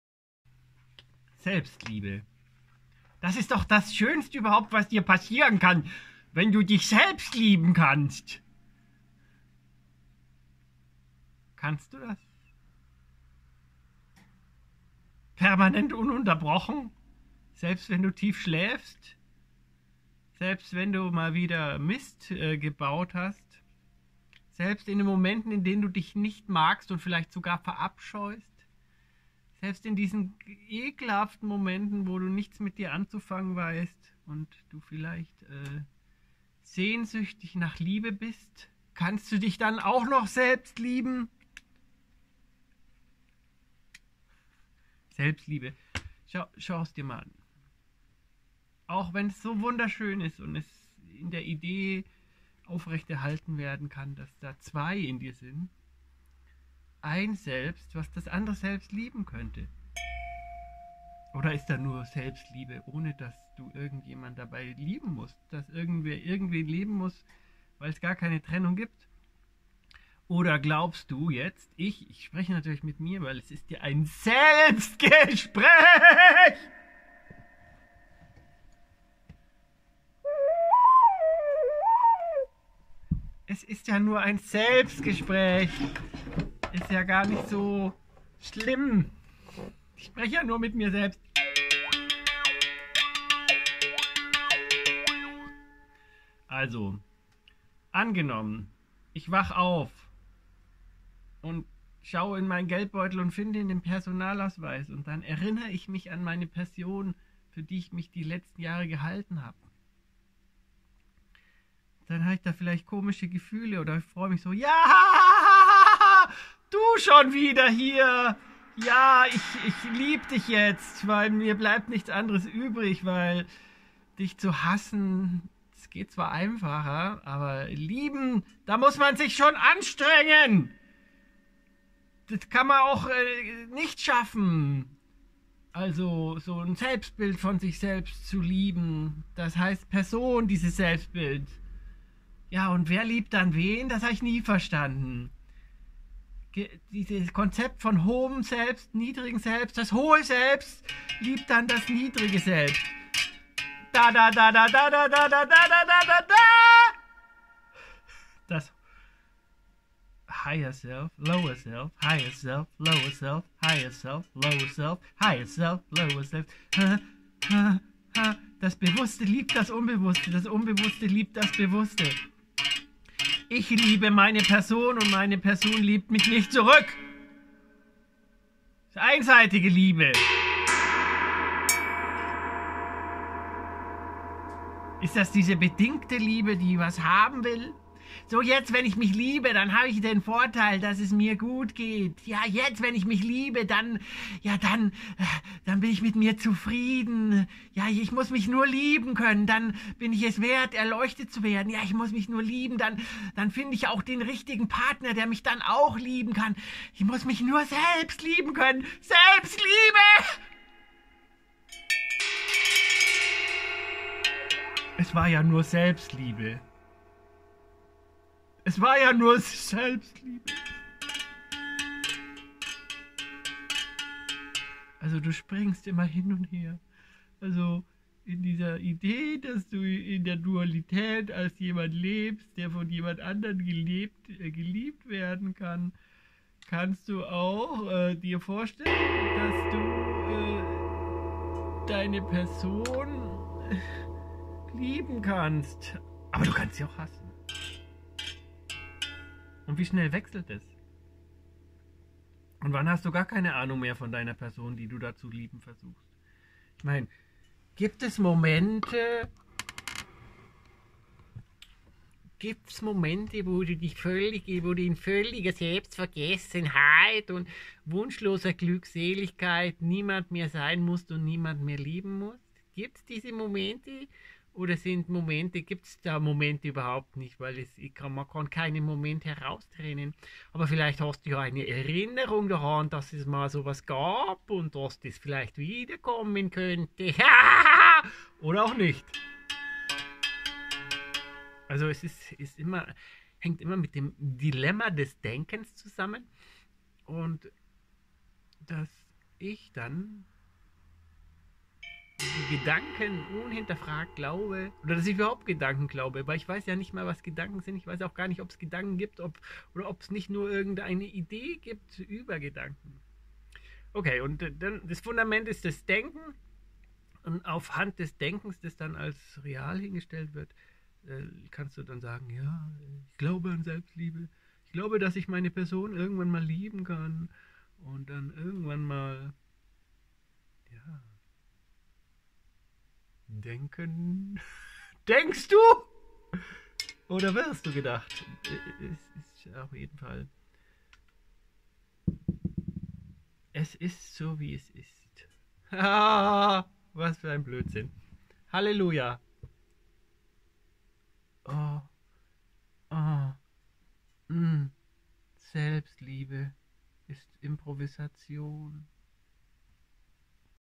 Selbstliebe, das ist das wunderbarste überhaupt
Nur ist mir völlig unklar, wer da wen lieben kann, ich mich selbst oder das selbst mich oder ich die Person,,die ich für einige Zeit als meine zu erfahren glaube oder kann ich es der Person überlassen mich zu lieben. Hahaha das für ein verwirrter Spaß mit der Selbstliebe.